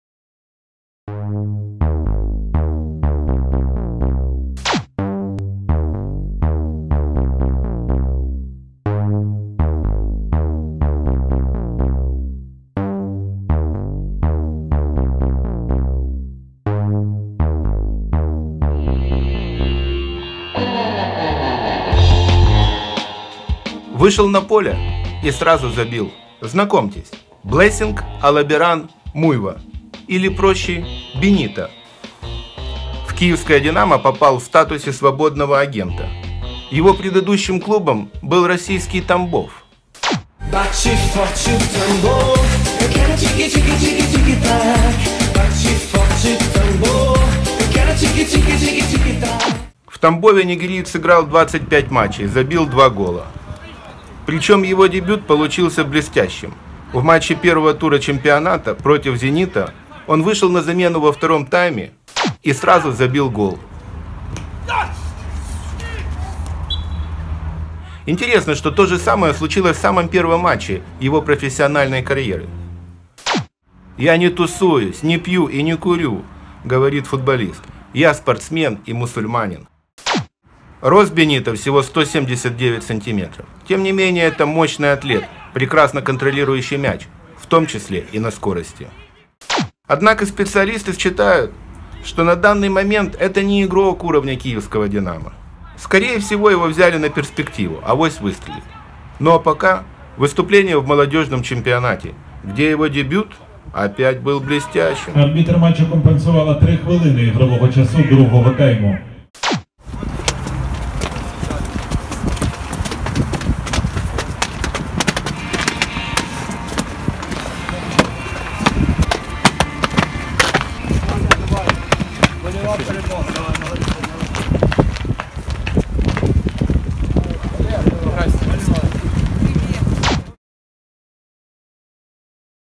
БЕНИТО : ПАРЕНЬ ВЫШЕЛ - ПАРЕНЬ ЗАБИЛ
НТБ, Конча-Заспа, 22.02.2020
Новичок киевского "Динамо" Бенито, дебютировал в матче молодёжных команд и сразу забил гол

Вы можете помочь каналу, переведя деньги на карту ПриватБанк:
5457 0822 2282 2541